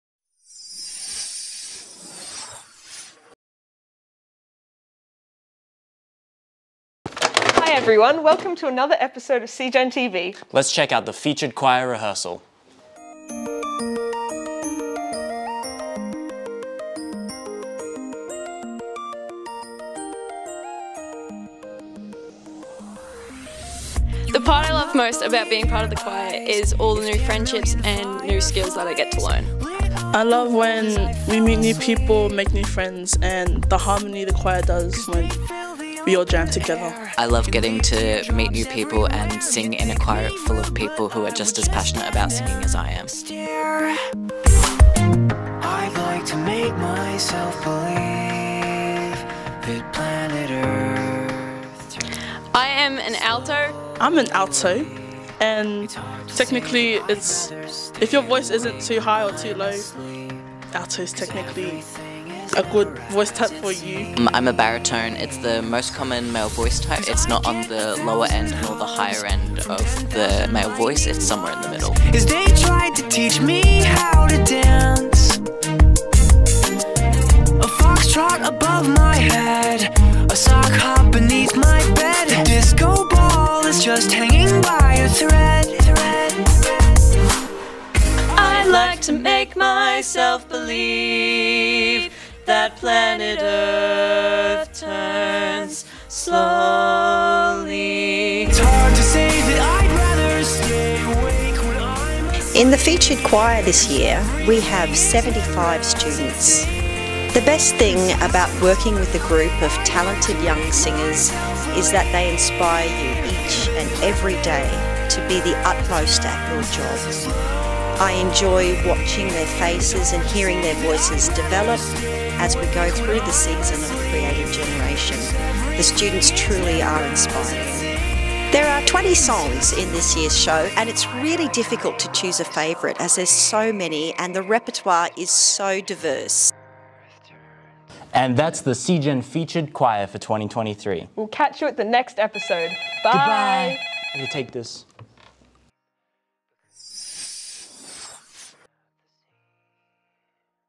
CGEN TV 2023 - Episode 7 - Meet the 2023 Featured Choir
Come behind the scenes as the media crew captures the awe-inspiring harmonies of our featured choir rehearsal. Don't miss this glimpse into the making of our CGEN featured choir performance! 🎶✨🧡